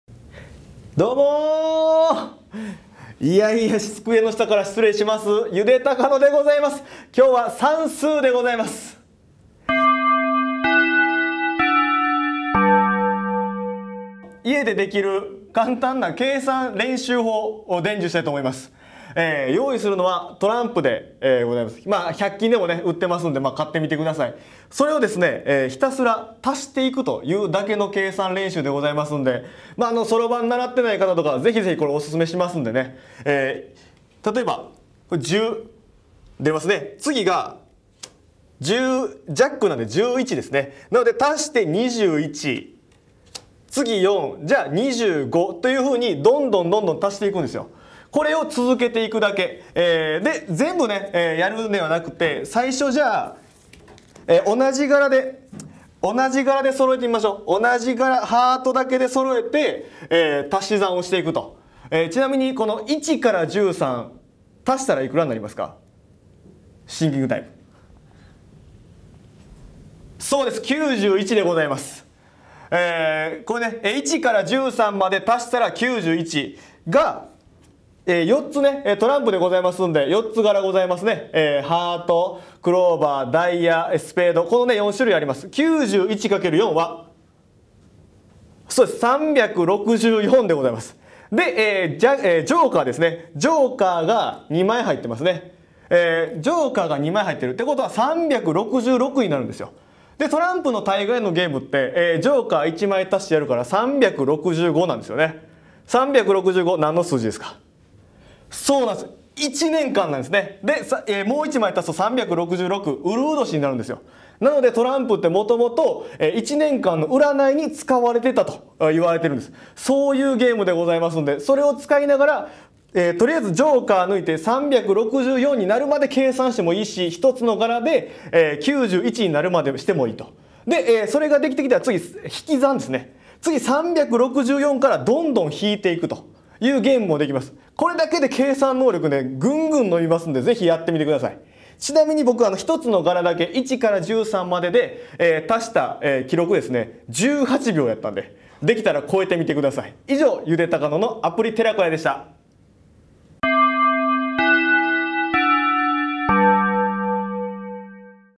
長野県住みます芸人ゆでたかの【アプリ寺子屋】テーマは算数「トランプを使って計算力をUPさせよう！」くだらなくて、役に立つ!休校中の皆さんを元気に!!芸人さんならではのスペシャル動画
休校中でご自宅にいる子どもたちやご家族向けスペシャル動画【アプリ寺子屋】
２代目長野県住みます芸人「ゆでたかの」さんが、くだらなくて、役に立つ!そんな講義を展開します。
算数で頭の体操！

トランプって何枚あるでしょう？

これは、カンタンだよね。

で、このトランプつかって
「地アタマ」よくする、計算特訓！

動画内の情報は「※ゆでたかの調べ」によるもので、諸説あります。